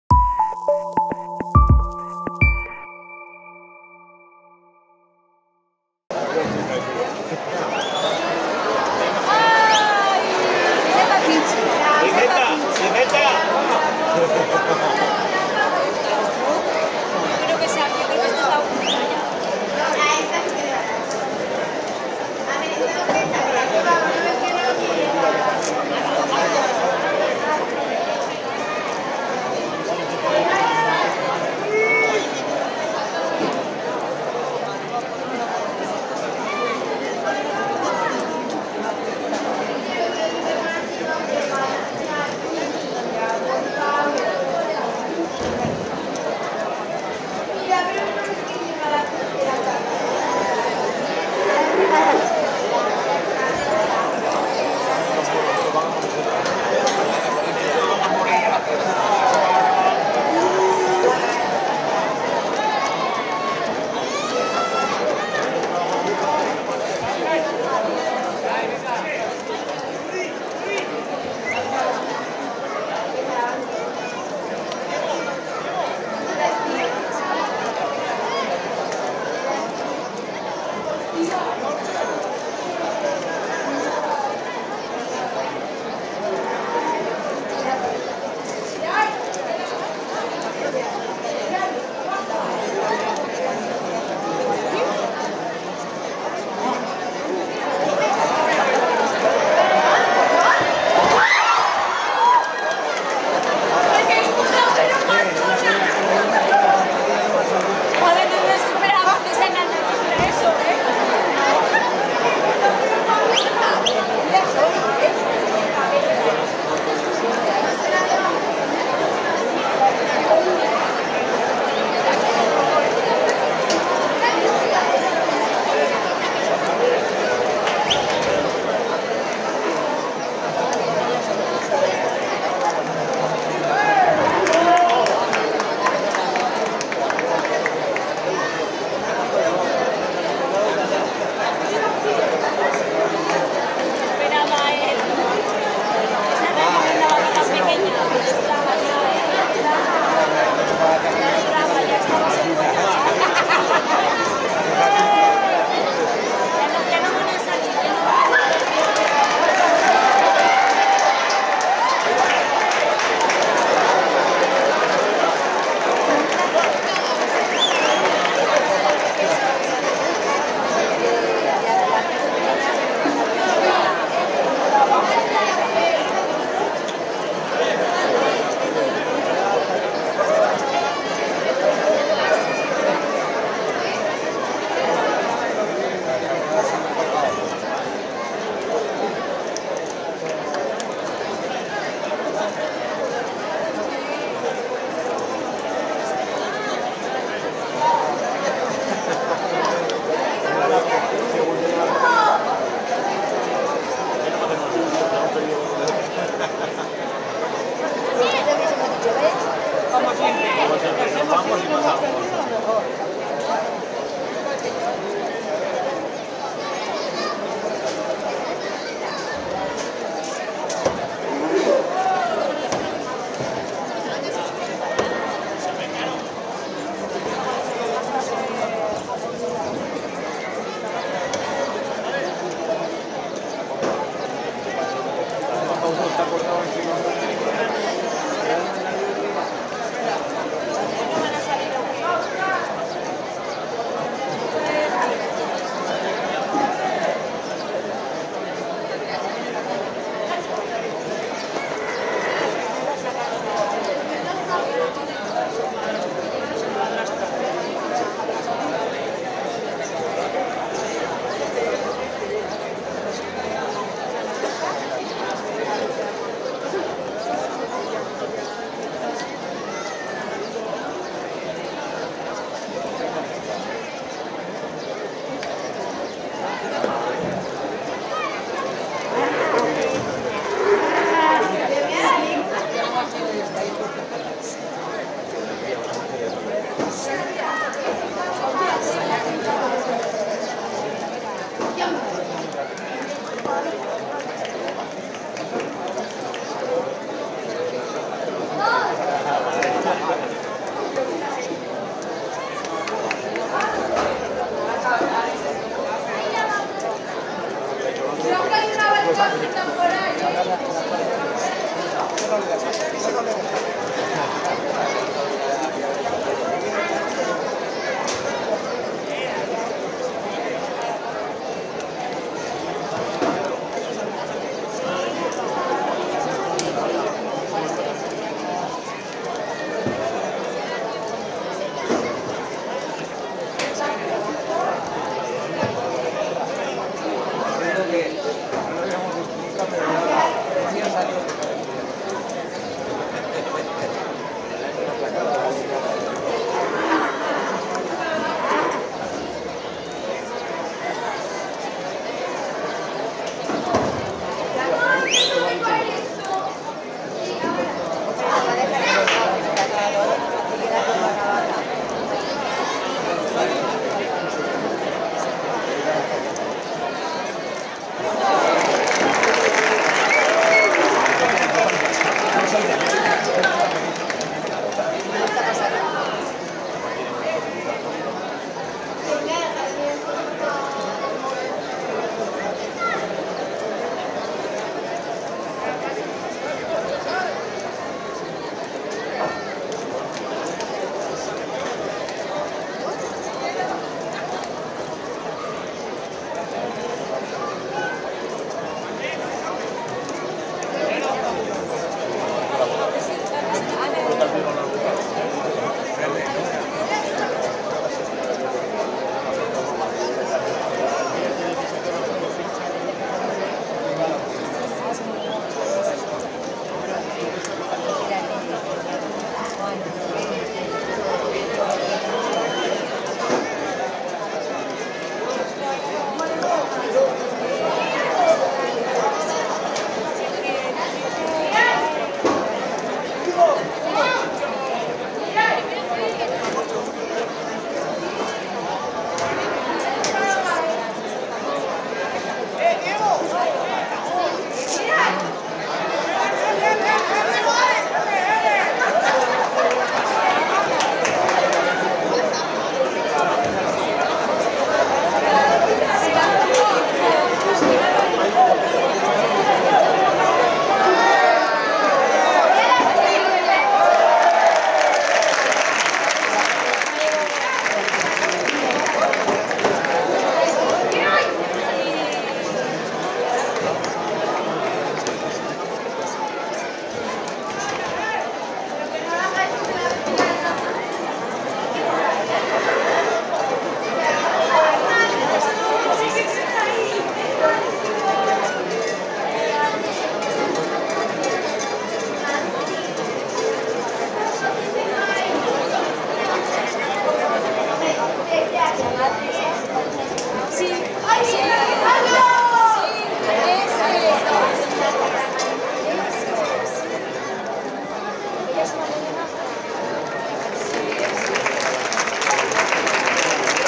Fiestas de primavera - Vaquillas - XI/XII (Cascante 2022)
Vaquillas en la plaza de toros con la ganadería José Arriazu e hijos (Ablitas)